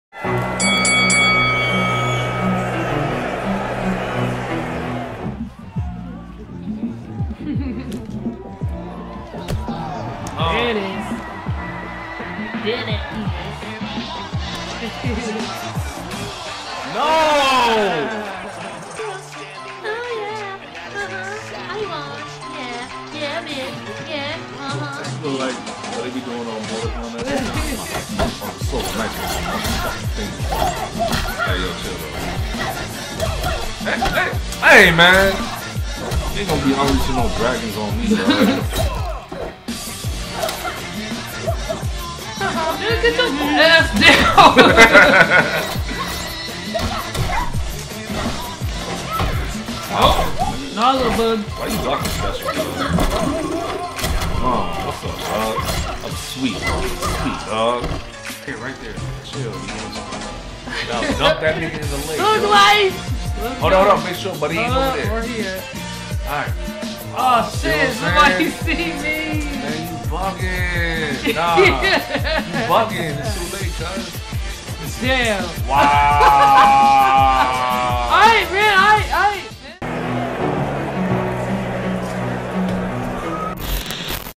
Welcome to Couple's Fight Night! NessaKwit Cast!
Welcome to Couple's Fight Night! Instead of taking our anger out on each other, we pick up the sticks! Enjoy!